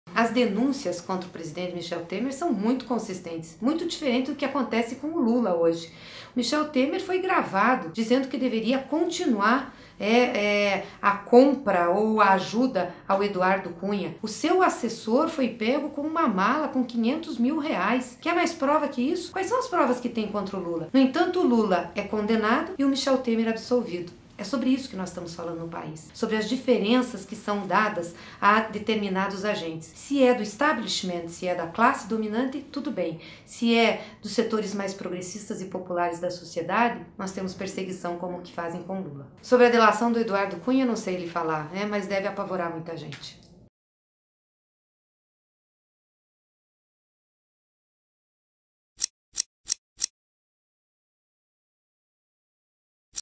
Gleisi Hoffmann: há mais provas contra Temer que contra Lula
A presidente nacional do PT, senadora Gleisi Hoffmann (PR-PT), concedeu entrevista ao Poder360 em 3 de agosto de 2017.